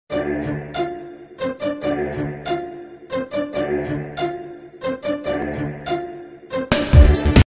Mortal Kombat diorama (The Temple stage).
Mortal Kombat hand-made diorama. The Temple stage from Mortal Kombat 3.